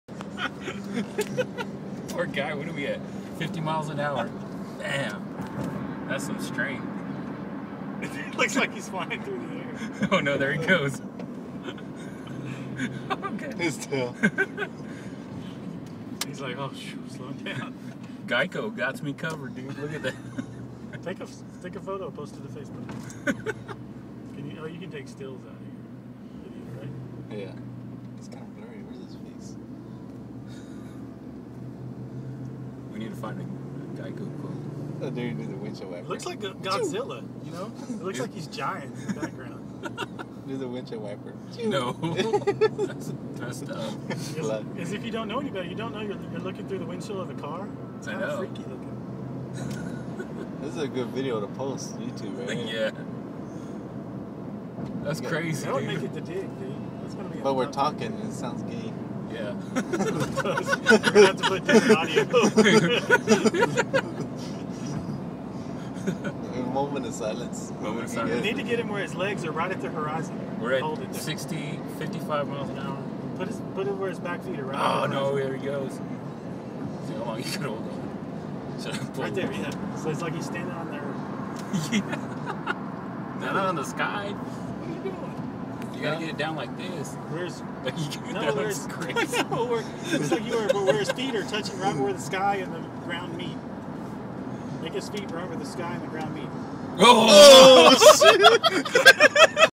Lizard holding on for dear life!
The lizard did it's best shot of holding on to the windshield.